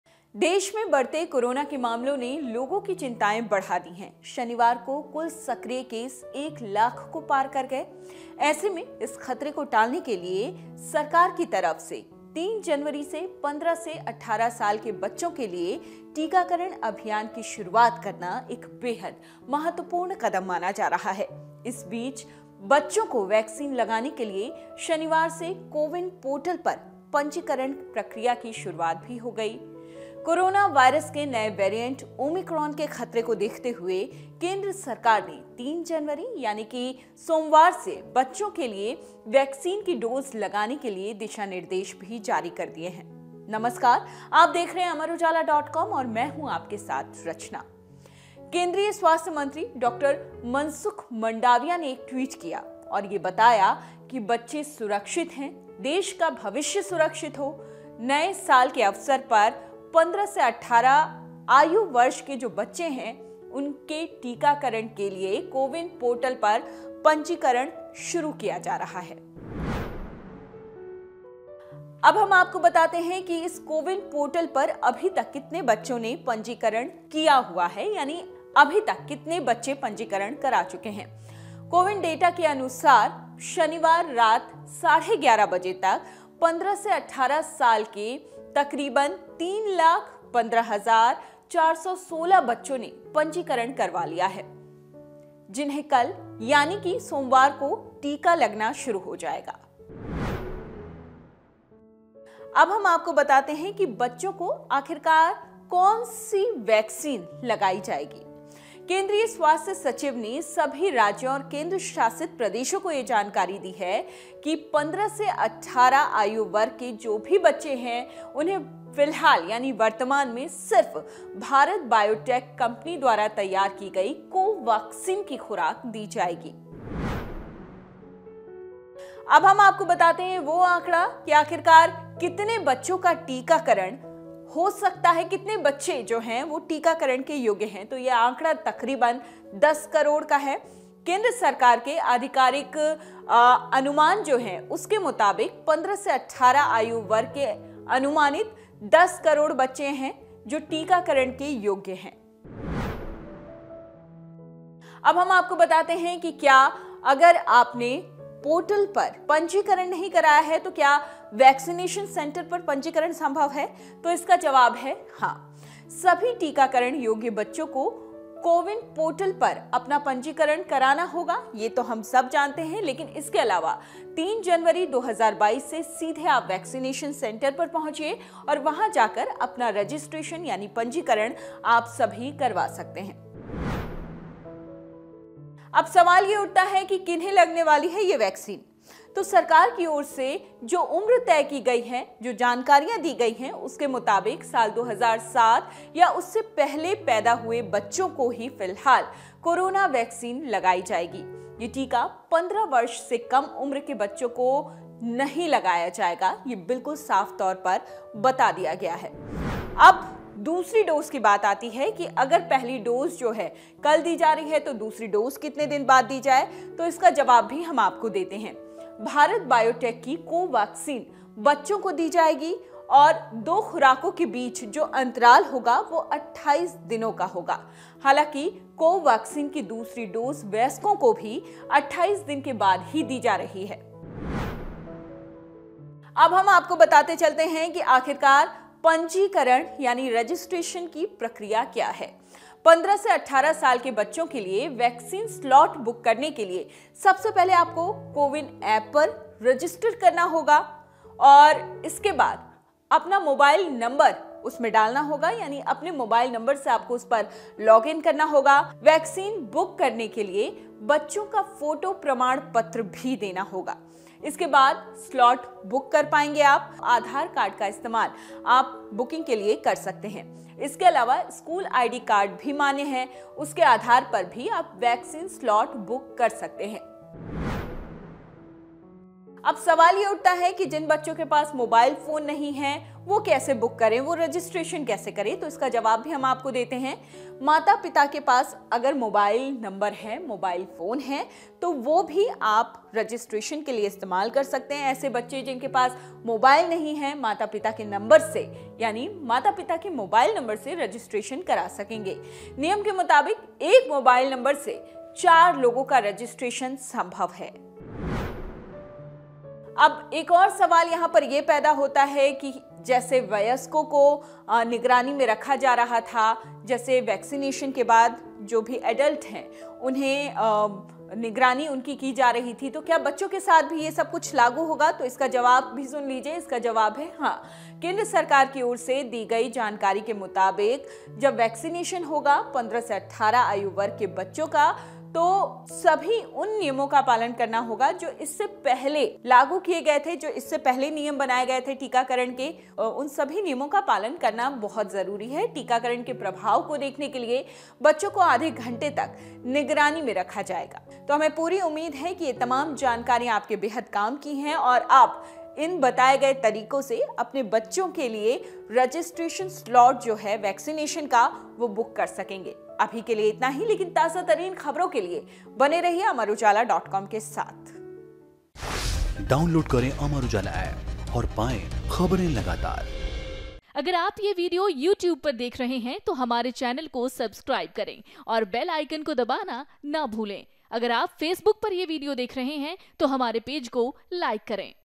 सोमवार से 15-18 साल के बच्चों को लगेगी कोरोना वैक्सीन | Corona Vaccine For Children | Omicron
Corona Vaccine अब 15 से 18 साल के बच्चों को भी दी जाएगी। इसे लेकर पीएम ने पहले ही एलान कर दिया था। बच्चों को वैक्सीन लगवाने के लिए कैसे कर सकते हैं पंजीकरण, देखिए। Registration For Corona Vaccine For Children